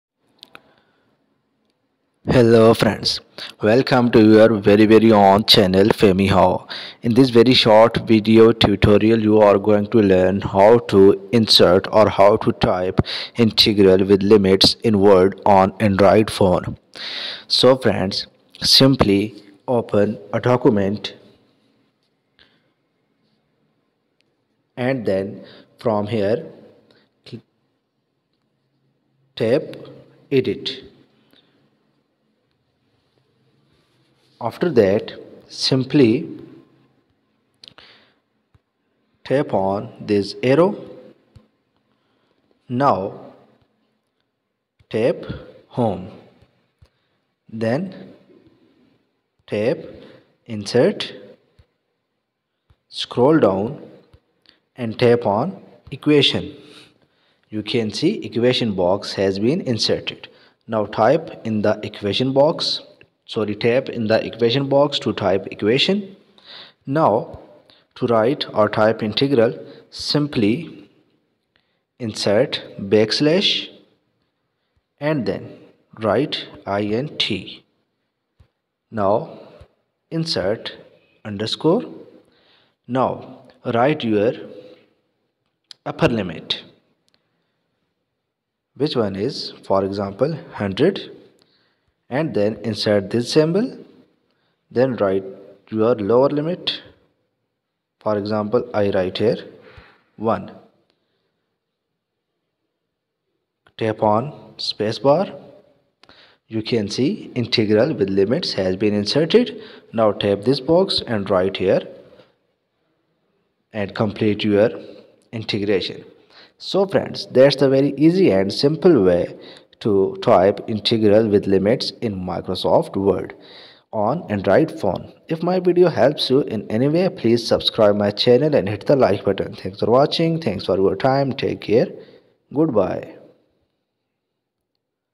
How to Type Integral with Limits in Word on Phone | How to Give Limits to Integral on Word In Mobile
This video tutorial will show you the tip and Trick to type Integral with limits in Word on Android Phone. After watching this video tutorial you will be able to give limits with integration in word on mobile phone.
This video tutorial may work in MS word in all Android phones.